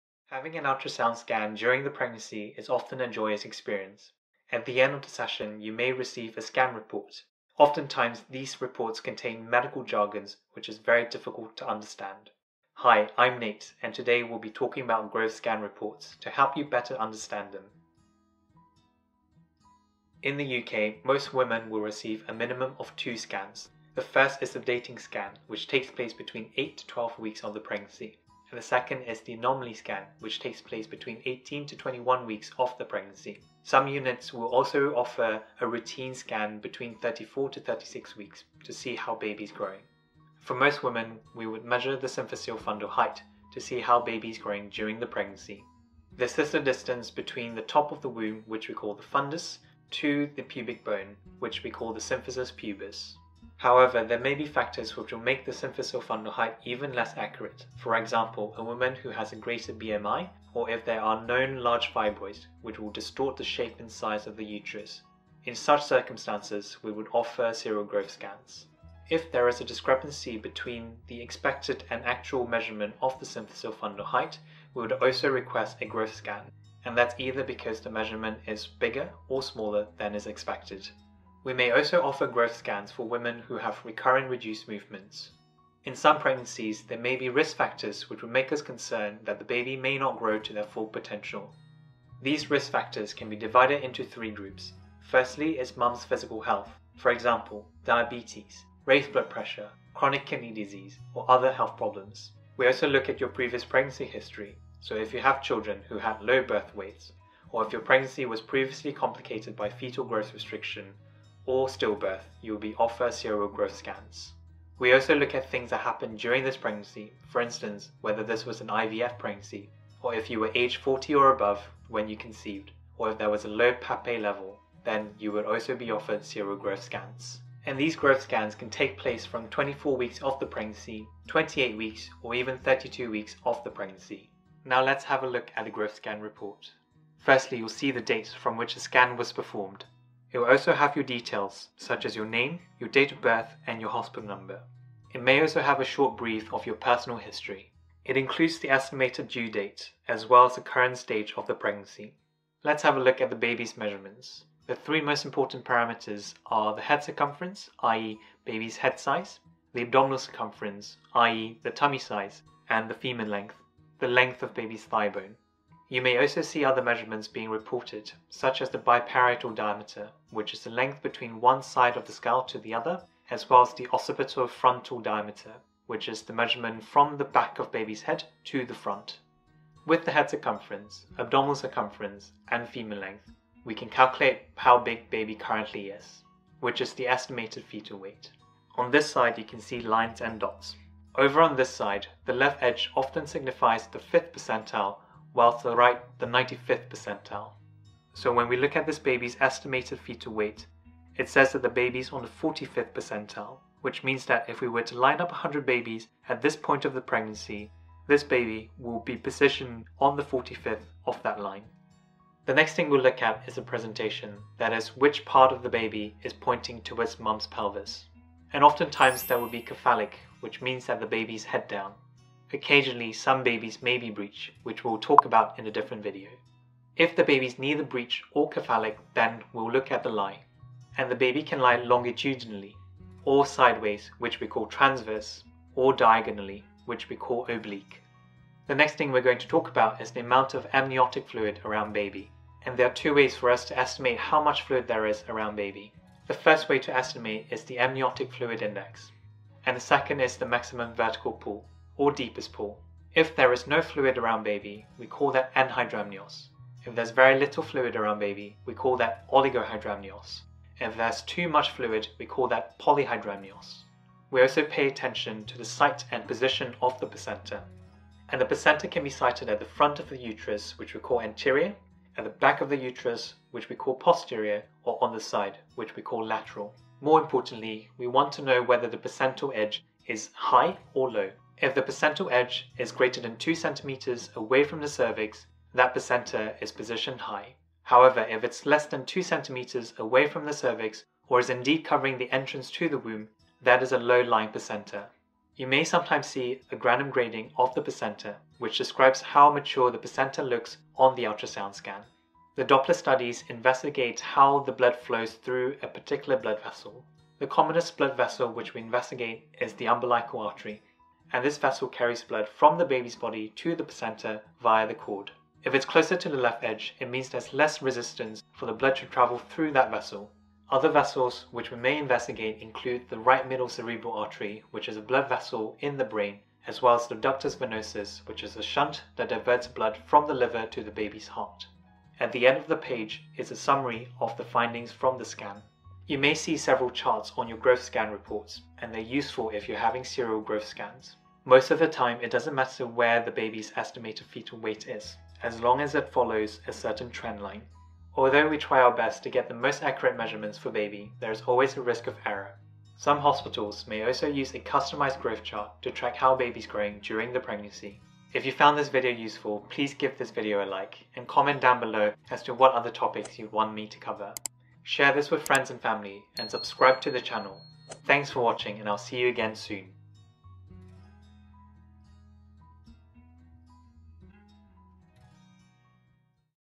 Growth Scans, Explained
Growth scans are common, especially if there are risk factors for a small baby.
But how can you make sense of what is written on the report?
This video will help you understand it better :)

***Ultrasound report used is fictitious and an homage to FRIENDS. Yes, I'm a 90s kid!!!

Ultrasound video by MART Productions.